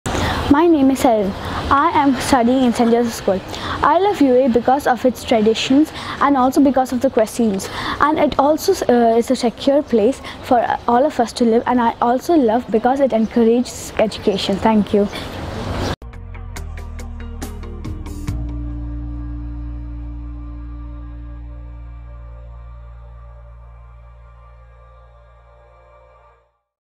My Love For UAE
Students from St. Joseph School, Abu Dhabi expressing their Love For UAE.